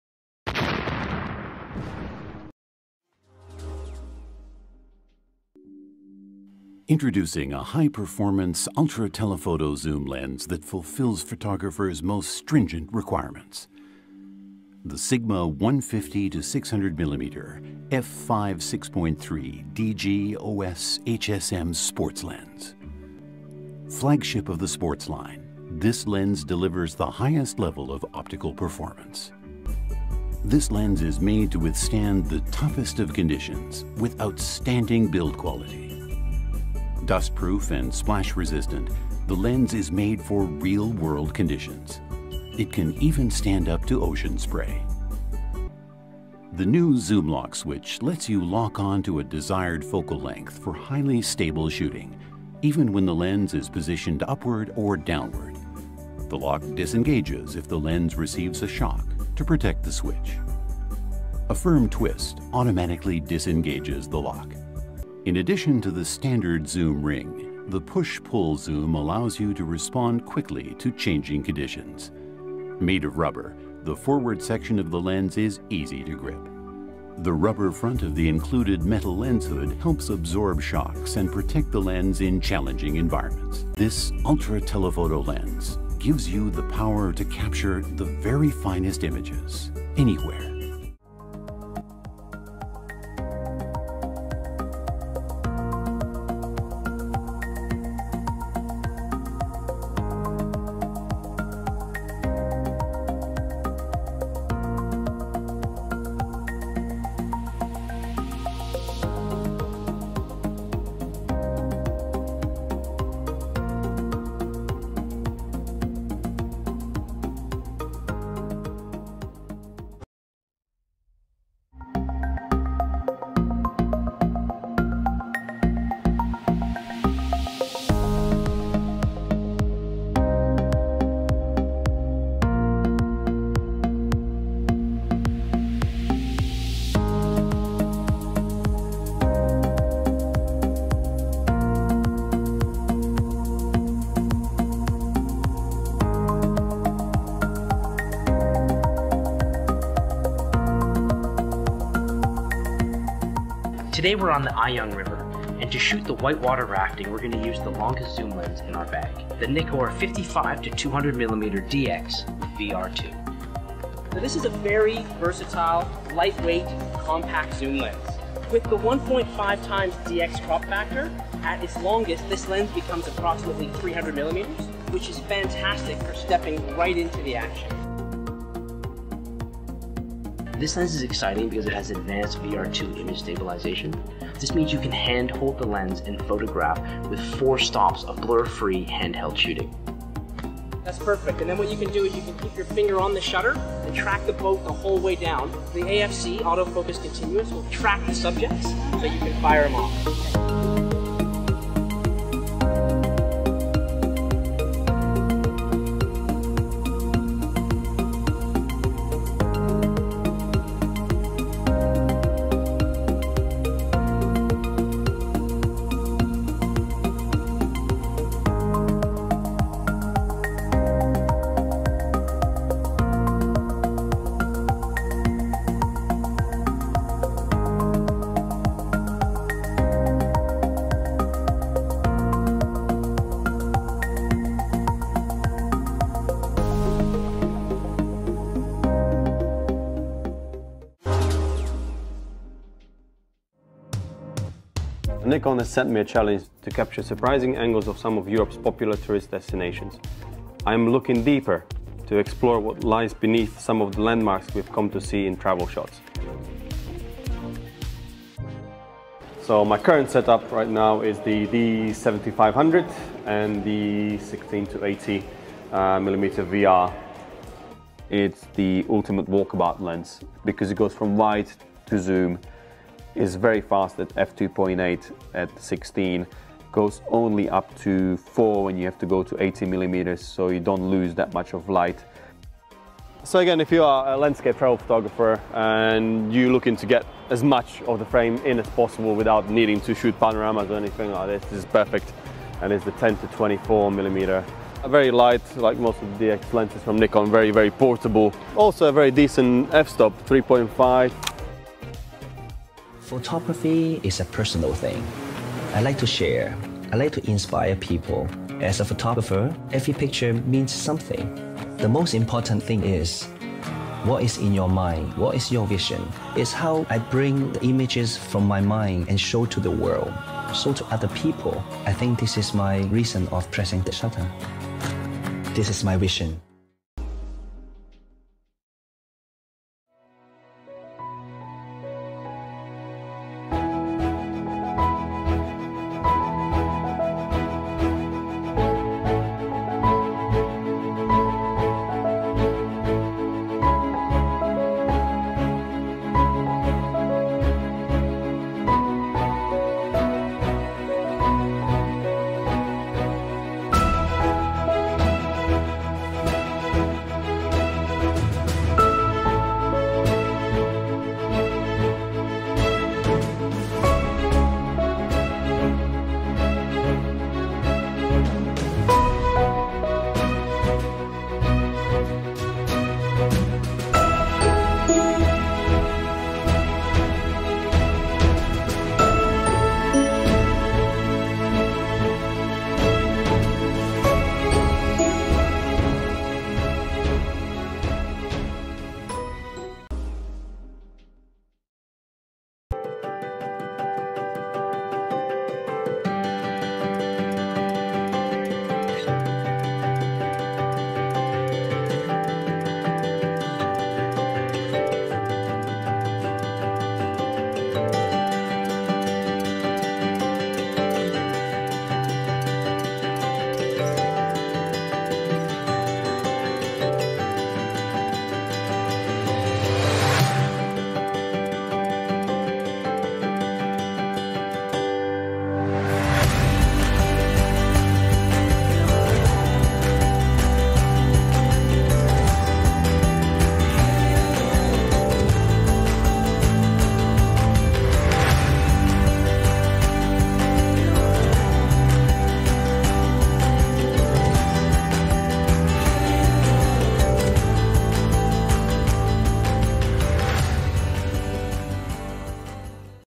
5 Best NIKON LENSES 2020 || Best budget Nikon Lenses
In today's video, we've hand-picked the 5 best Nikon lenses for 2020.

► 1. Sigma 150-600mm 5-6.3 Sports DG OS HSM Lens for Nikon
► 2. NIKON AF-S DX NIKKOR 55-200mm f/4-5.6G ED VR II Lens
► 3. Nikon AF-P NIKKOR 70-300mm f/4.5-5.6E ED VR Lens
► 4. Sigma 70-200mmF/2.8 DG OS HSM Lens for Nikon
► 5. Nikon AF-S DX NIKKOR 16-80mm f/2.8-4E ED